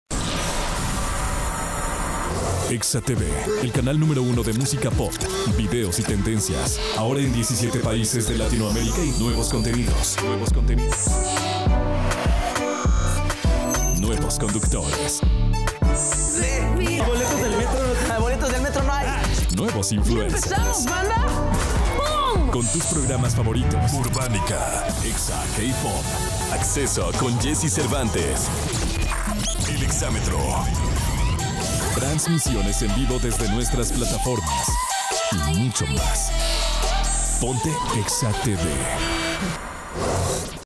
EXATV llega con NUEVA programación
Este 16 de marzo ExaTV 📺 estrena programación ¡Nuevos conductores, influencers, programas y ahora para TODA LATINOAMÉRICA! 🙌